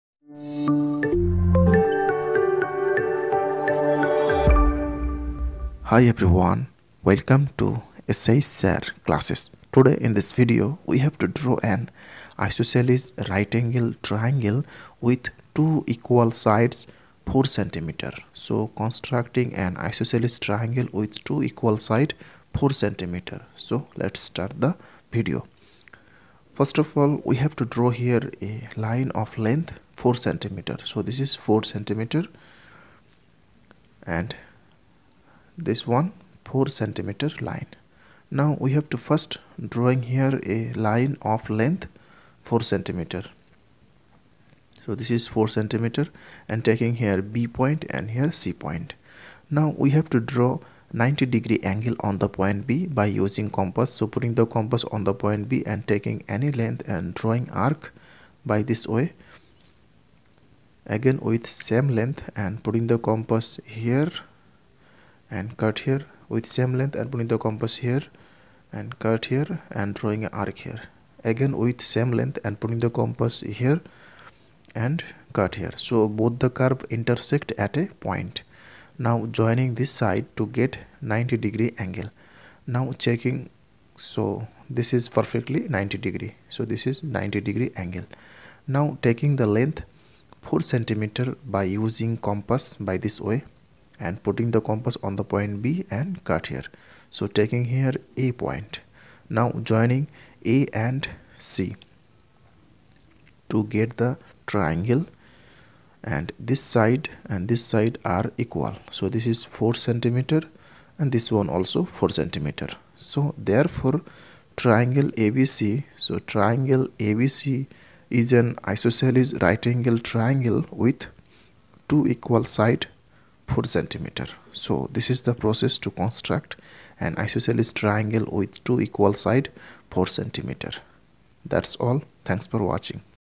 How to draw an isosceles right angle triangle with two equal sides 4 cm.shsirclasses.
In Easy Way how to draw an isosceles right angle triangle with two equal sides 4 cm.shsirclasses. Geometrical Construction.